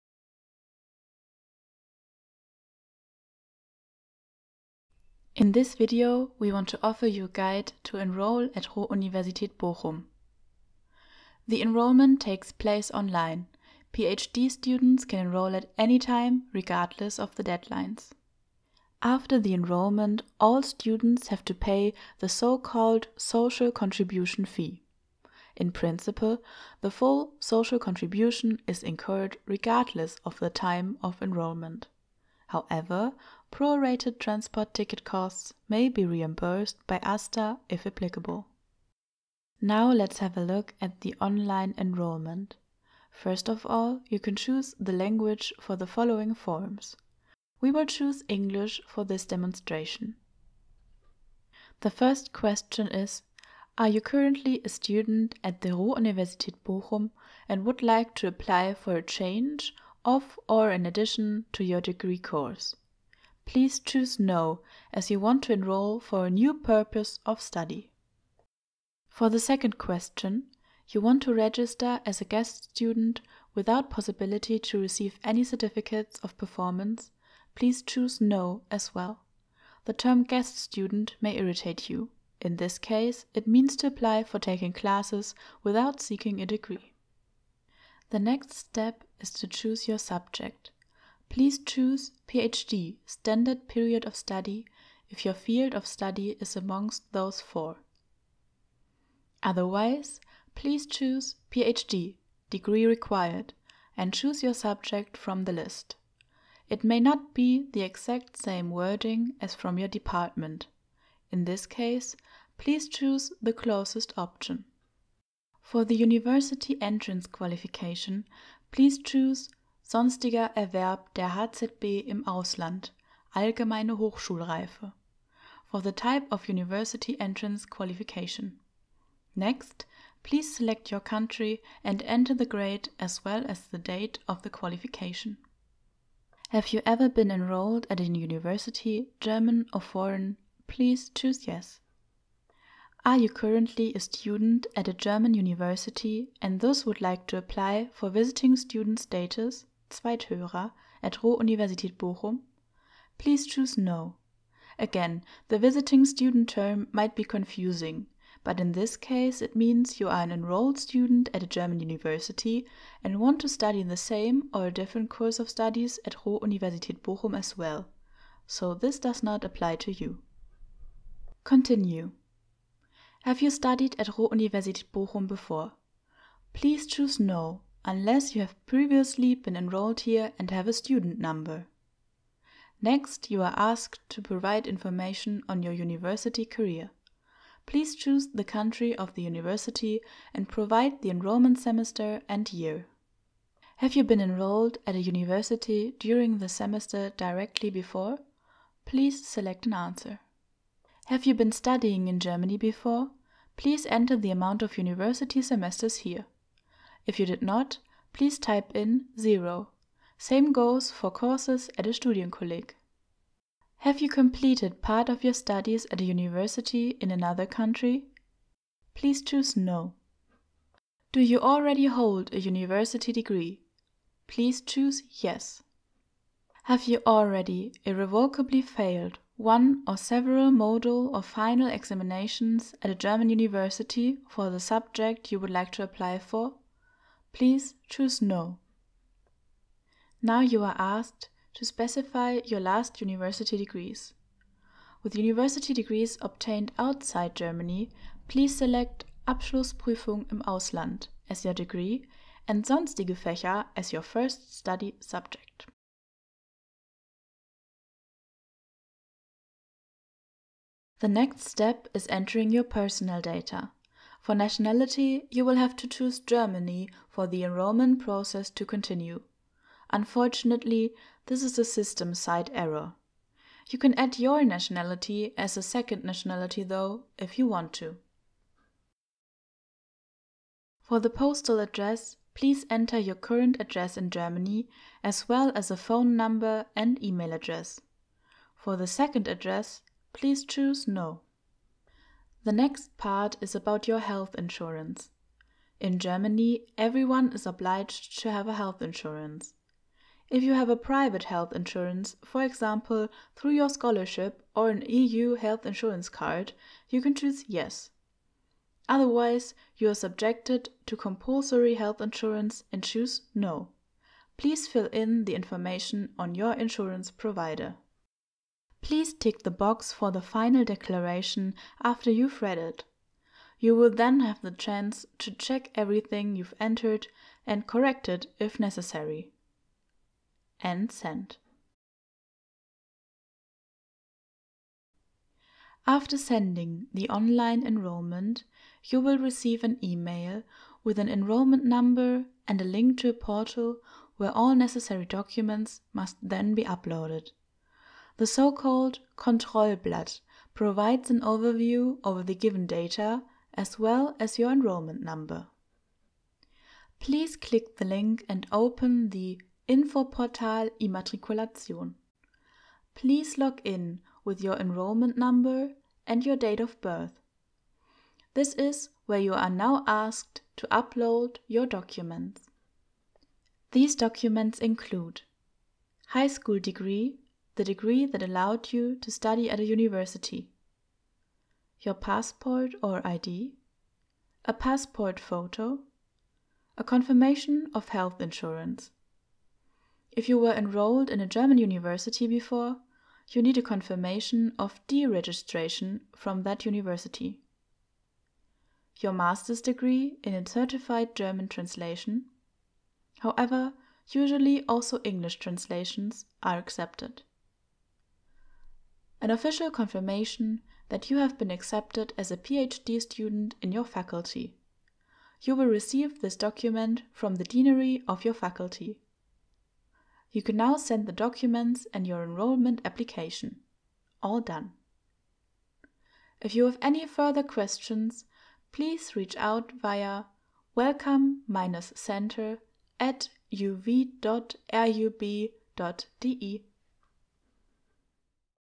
Online enrolment for PhD students at RUB
With this video want to guide you through the process of online enrolment at RUB.